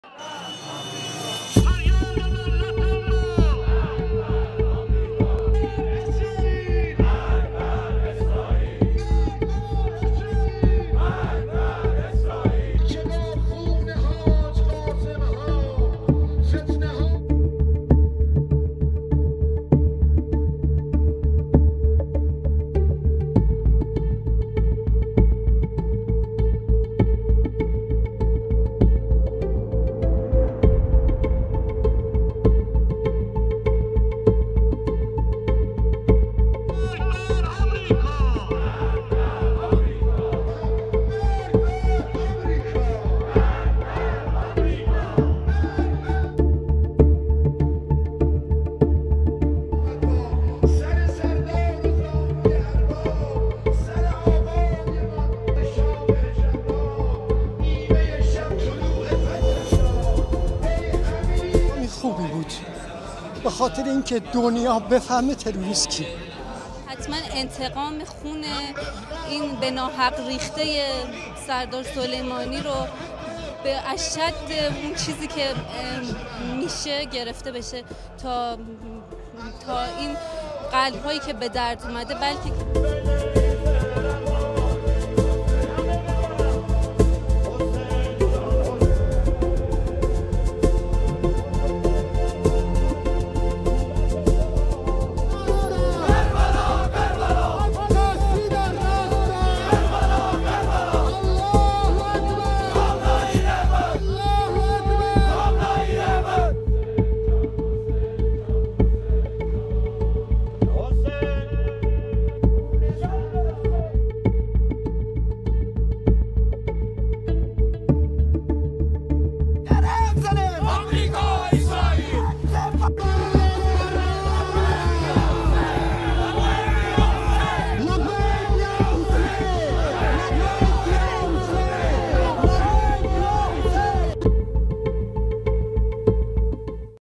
Milhares de iranianos pedem "vingança" pela morte do comandante Soleimani
O líder da Guarda Revolucionária Iraniana foi morto num ataque em Bagdad, ordenado por Donald Trump. Nas ruas de Teerão, milhares de manifestantes acusam os EUA de terrorismo e pedem "vingança".